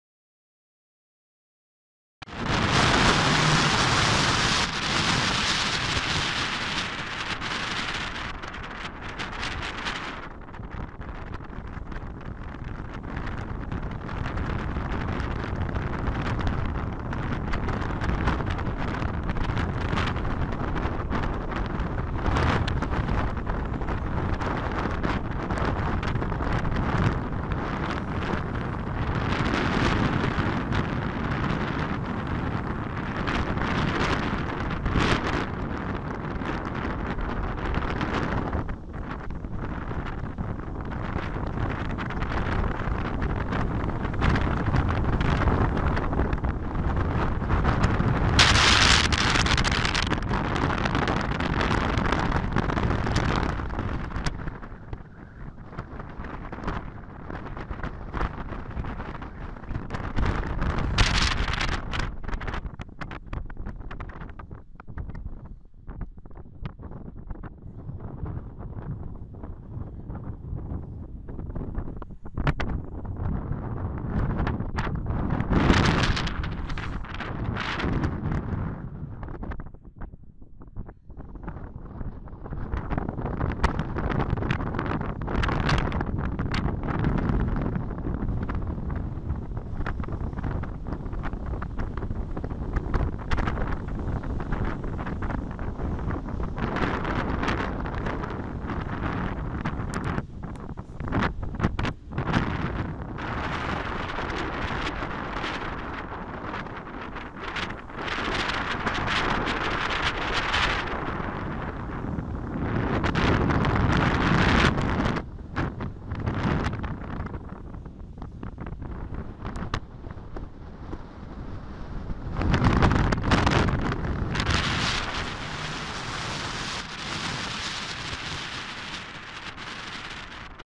Salto del Nervión 10/02/2017
Tras las intensas lluvias que se han producido en la zona norte de Burgos, el salto más grande de la Península Ibérica con sus 222 m de altura, crea un espectáculo natural único